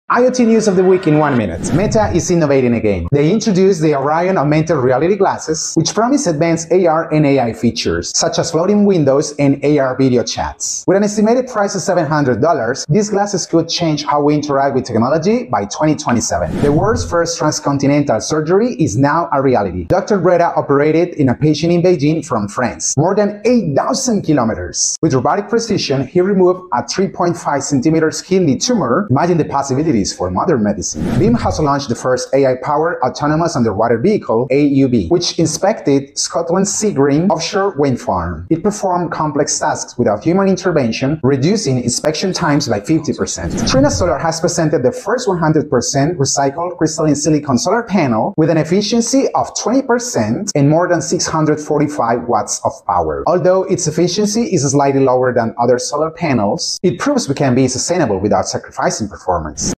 Tech news of the week in 1 minute.
New smart glasses by Meta, transcontinental surgeries, and more! Check out the latest news on technology, AI, and AR. Here at DeepSea Developments, tech news of the week in 1 minute.